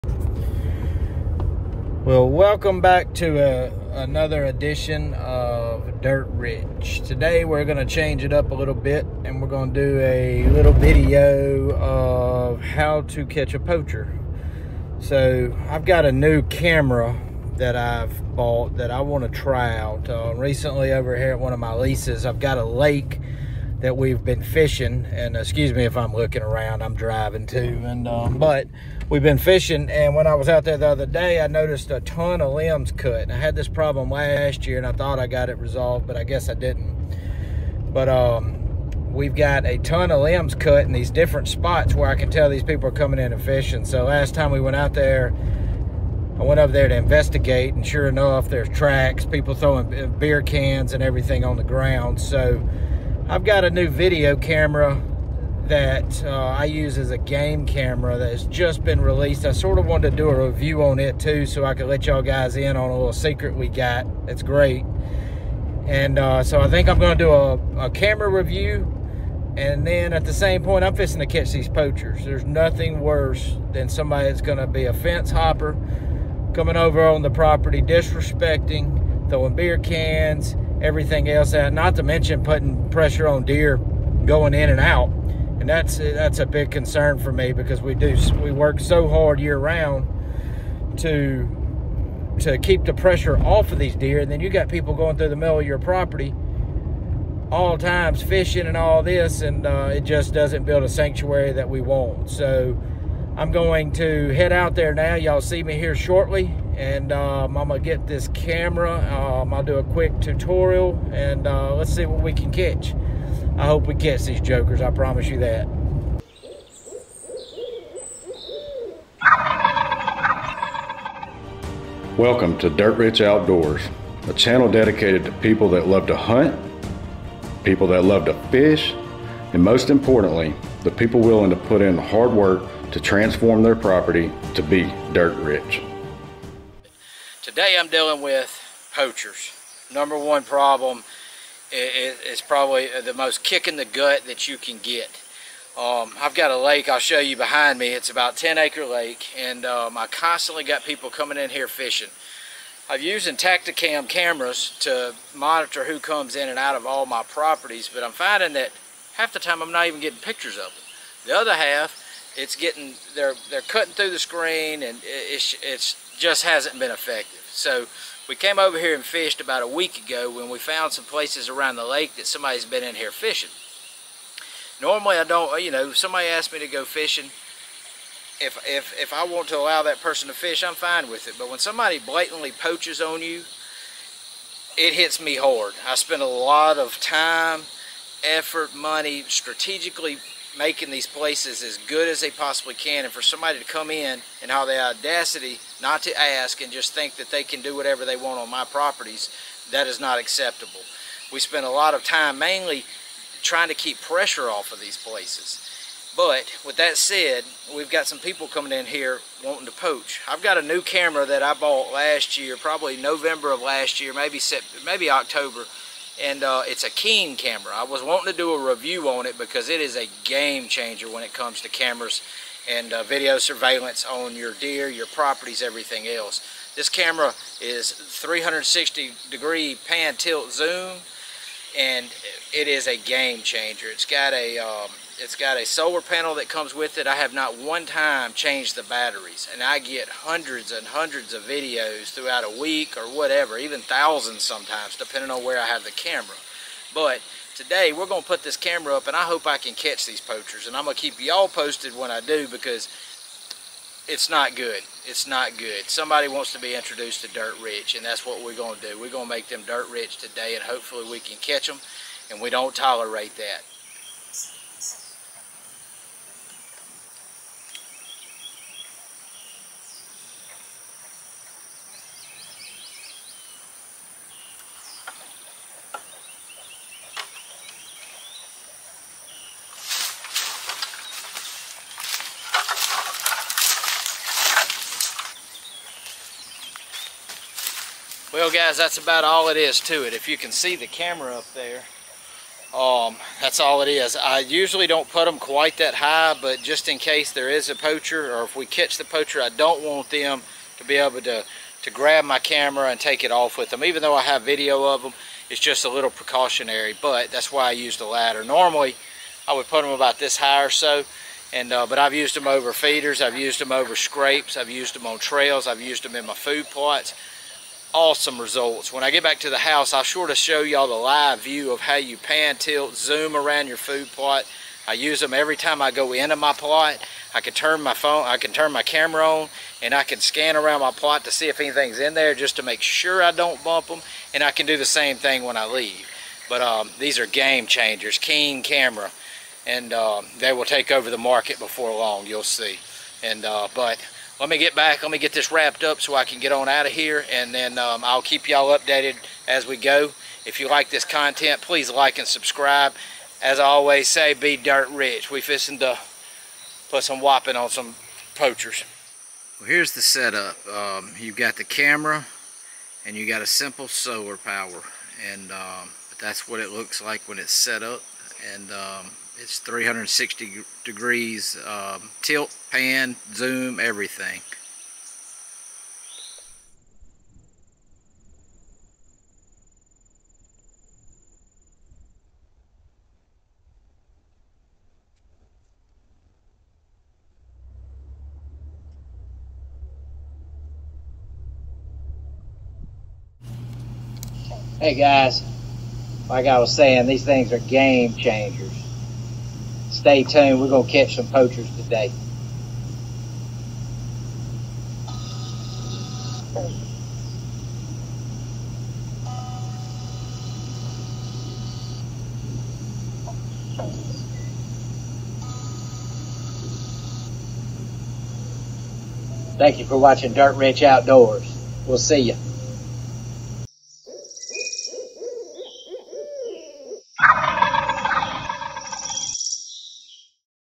CATCHING POACHERS - Using my KEEN Ranger PT Camera
Today we're setting up my KEEN Ranger PT Camera to catch poachers fishing and littering at our trophy bass lake!